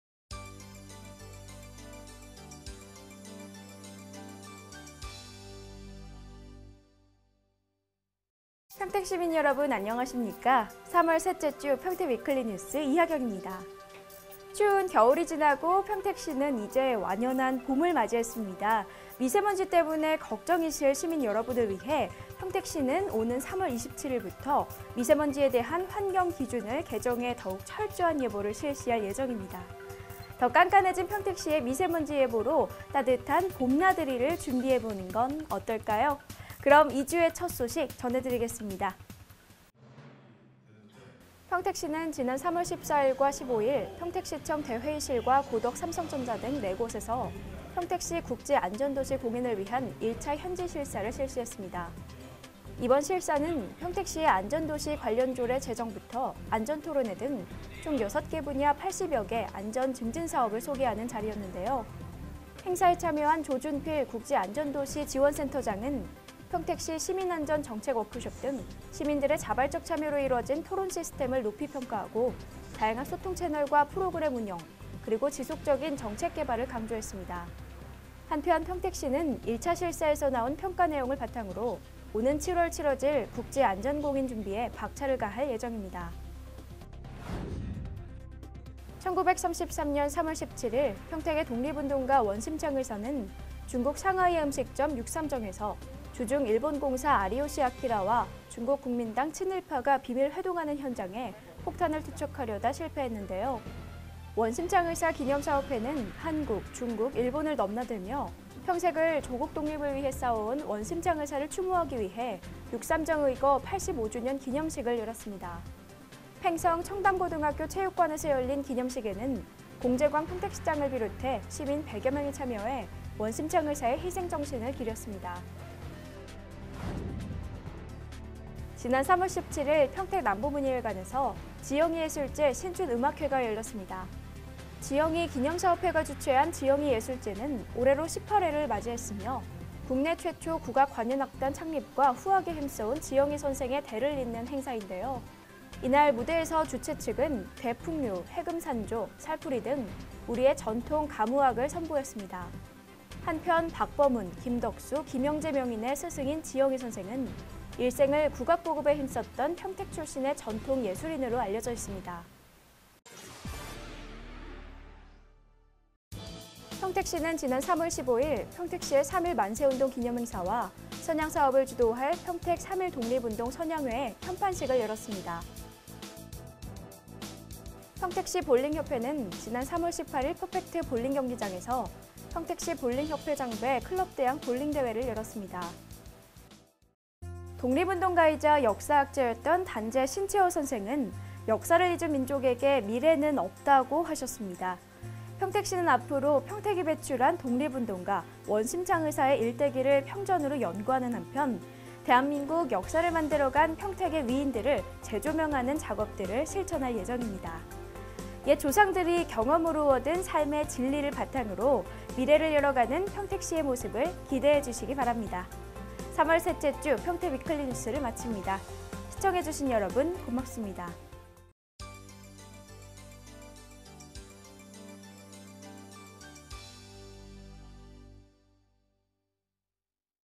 [희망톡톡평택TV] 위클리뉴스와 함께하는 3월 셋째 주
국제안전도시 공인1차 현지실사

육삼정 의거 85주년 기념식

지영희 예술제 신춘음악회